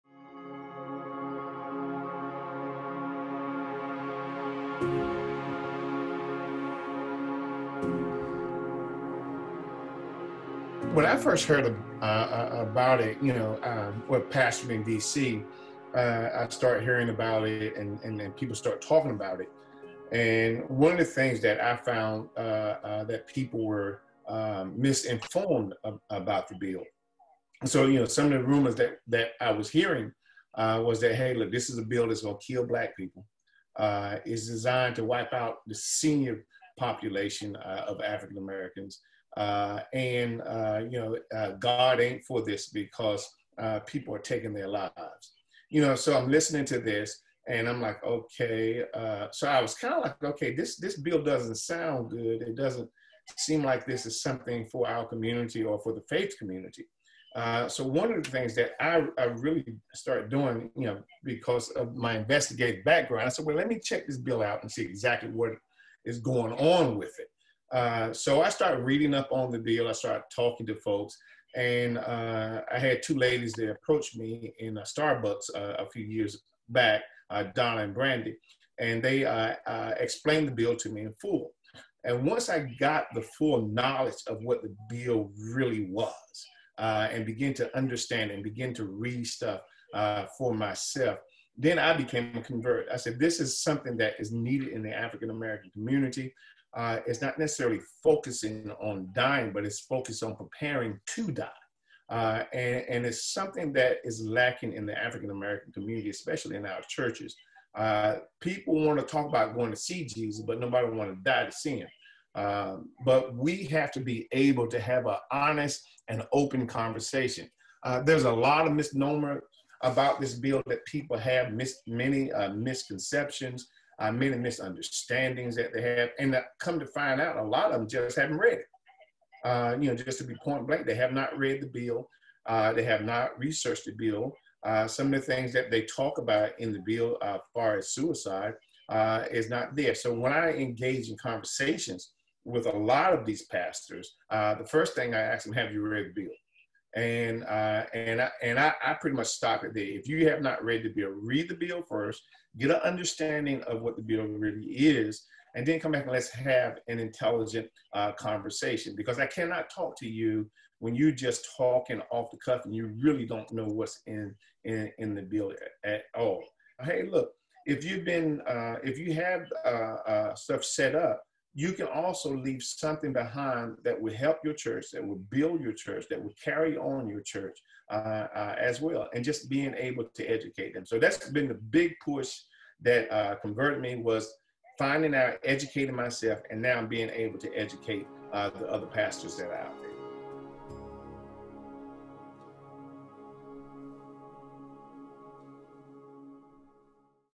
My Faith Journey on the Subject of Medical Aid in Dying
Rev. Charles McNeil discusses how his position on medical aid in dying evolved after encountering the work of Compassion & Choices.